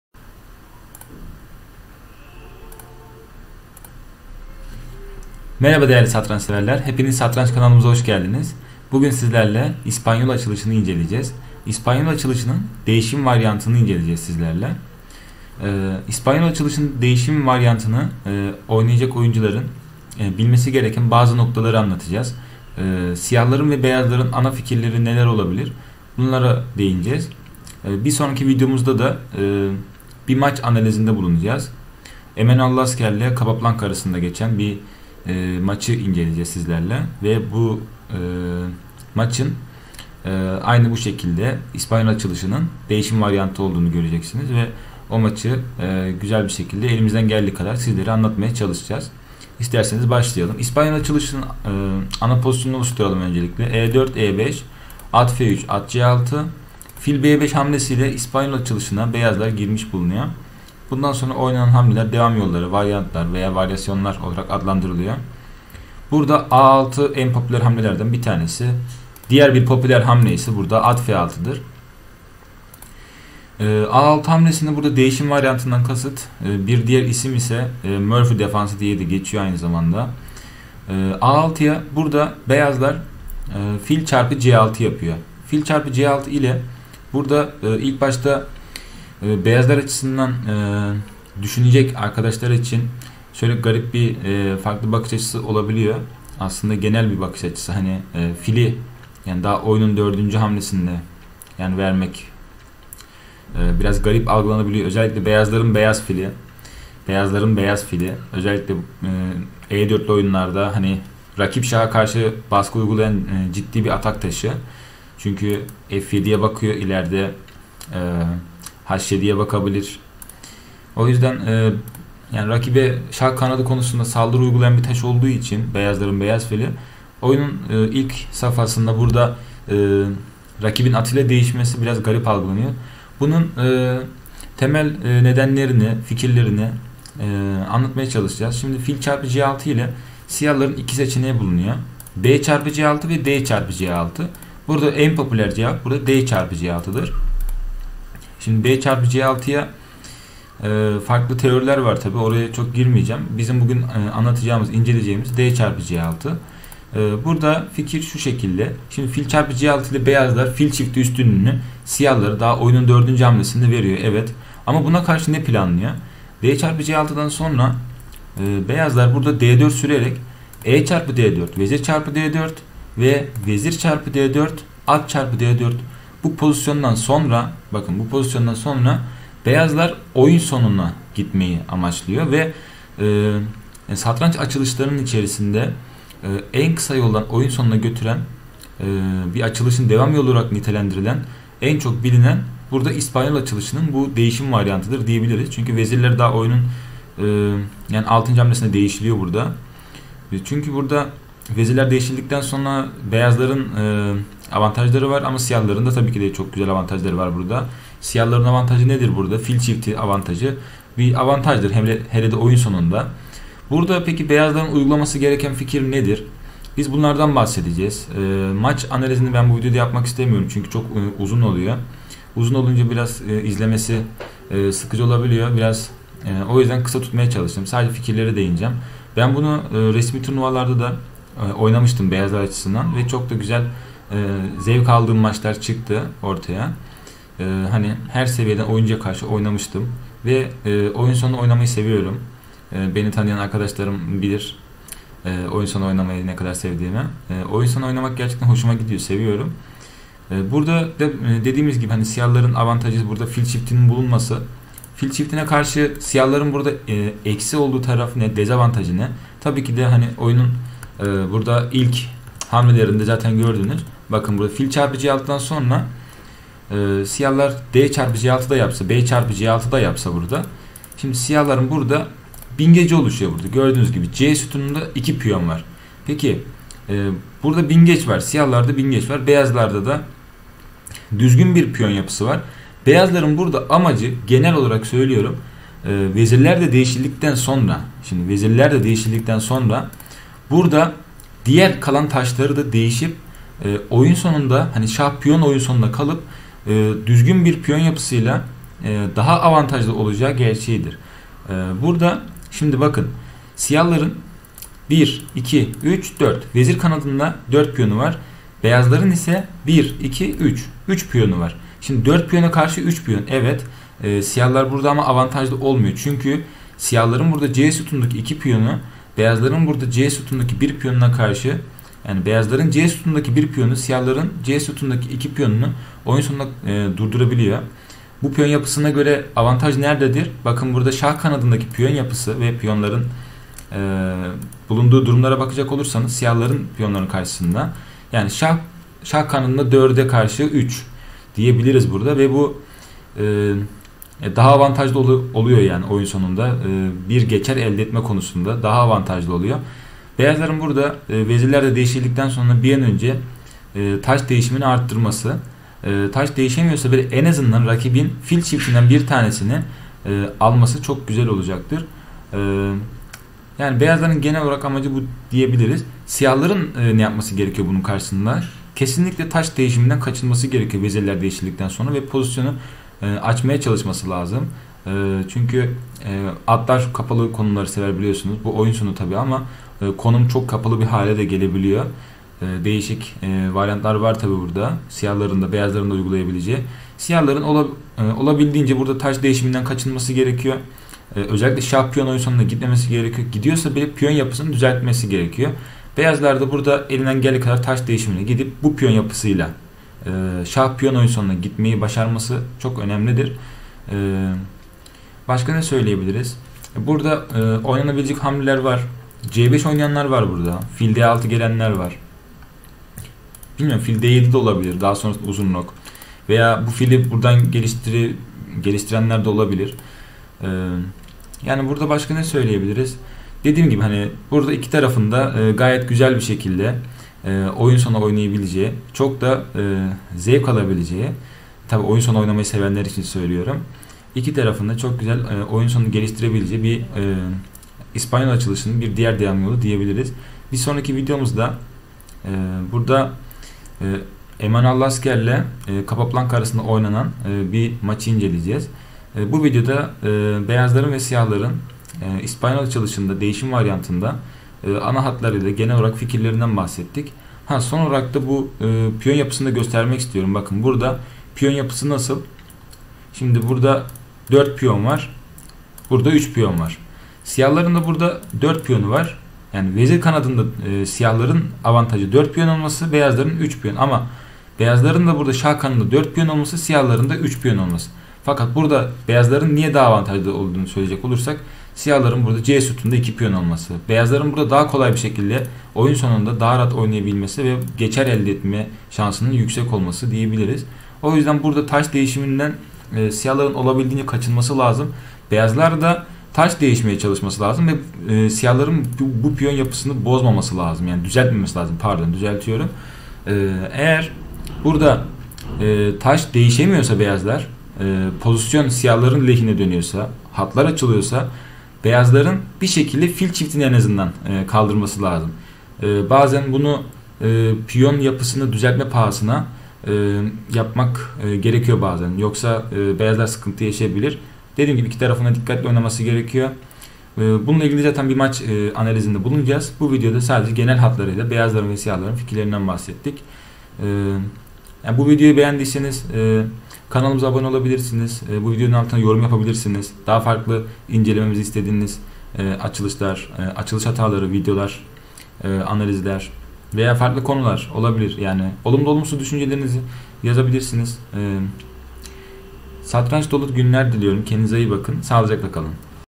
Online Satranç Açılışları: İspanyol Açılışı / Değişim Varyantı | Ana Fikirleri ve Devam Yolları
Online Satranç Açılışları serimizde Kuş Açılışı  ile karşınızdayız! Herkese satranç dolu günler diliyorum. :)